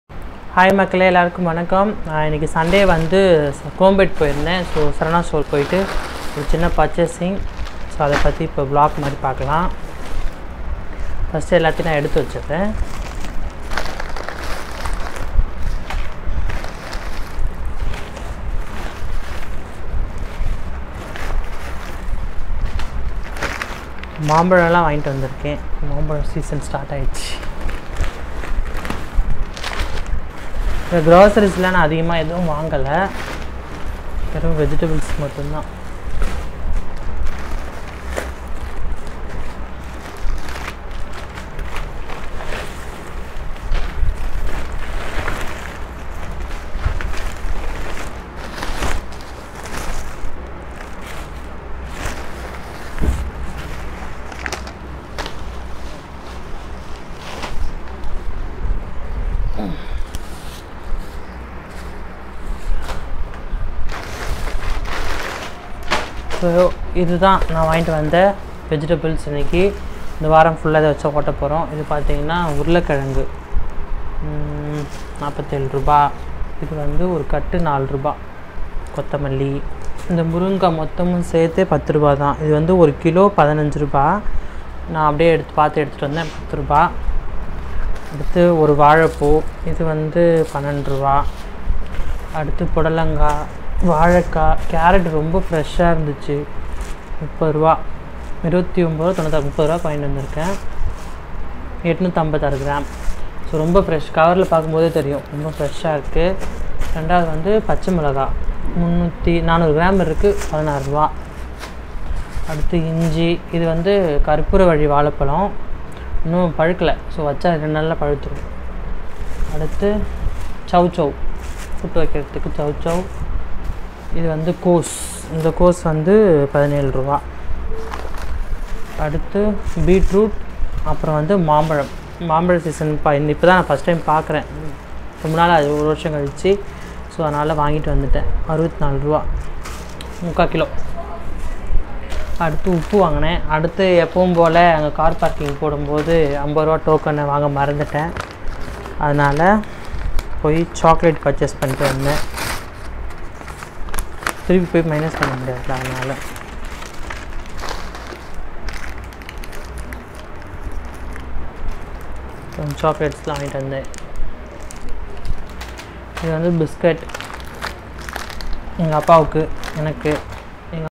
Chrompet saravana store grocery shopping vlog 😉🥬🥦🥕NAVAJAGI
chrompet saravana store grocery shopping Vlog in Tamil
grocery shopping on a budget
budget grocery shopping
grocery haul on a budget